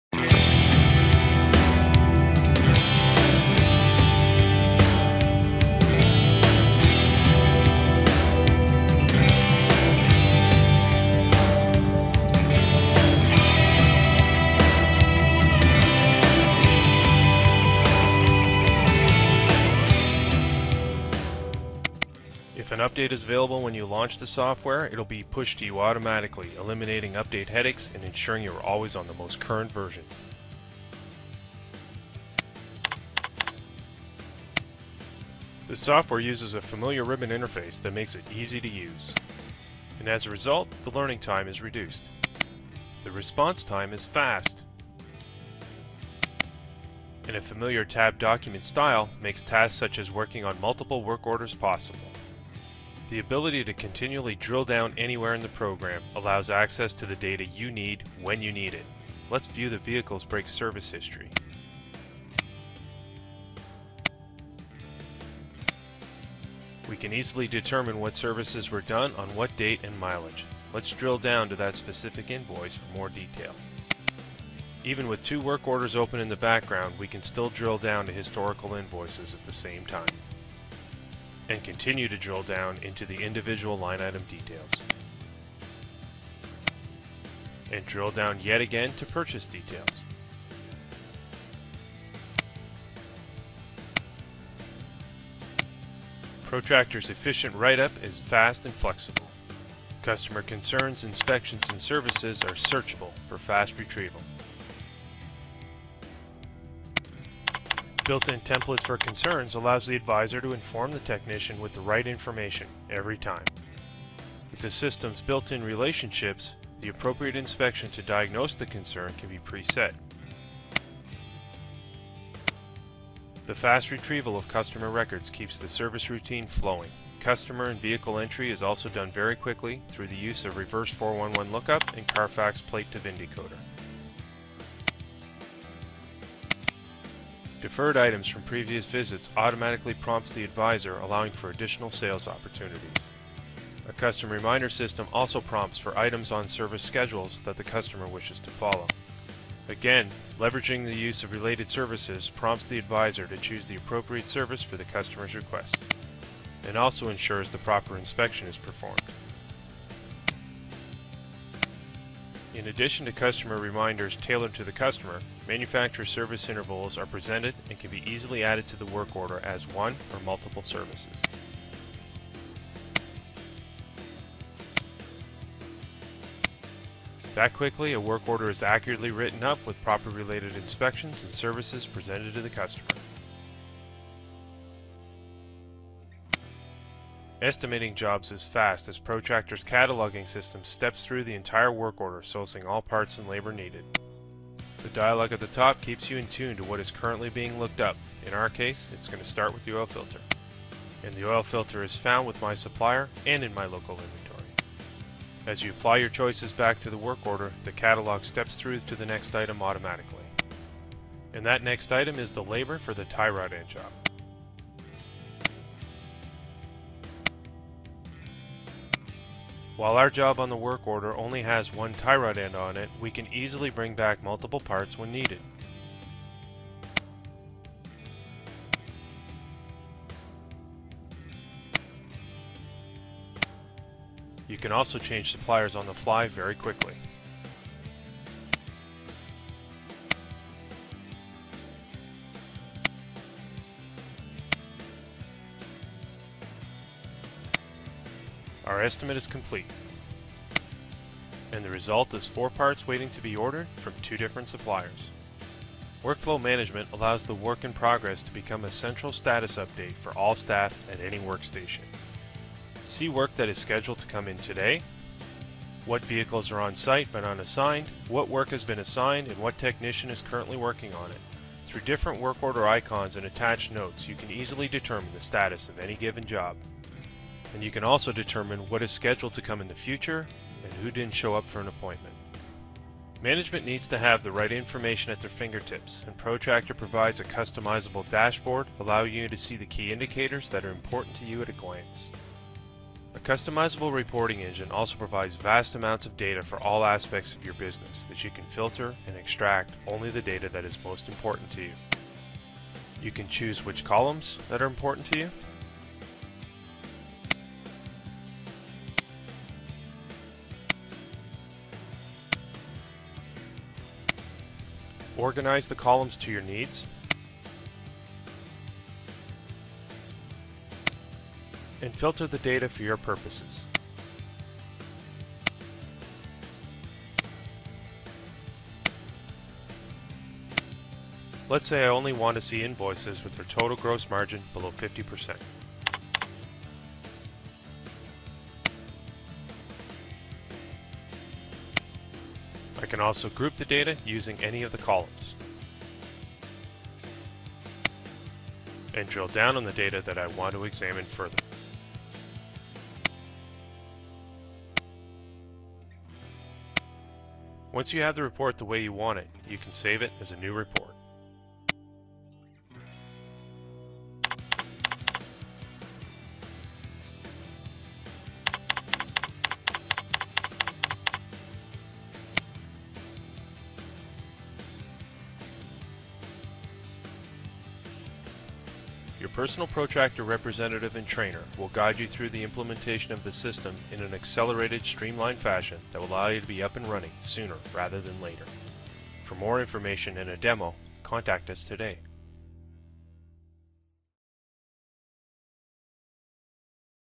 Efficient Write Up - Protractor Shop Management Software Demo
Watch this quick 6 minute tour of some of the key highlights of the Protractor software Program. From smart up-to-date Windows.NET based layout, cloud-technology that allows you to connect from anywhere to robust reporting capabilities, our program has the power to manage your business in an easy to use layout.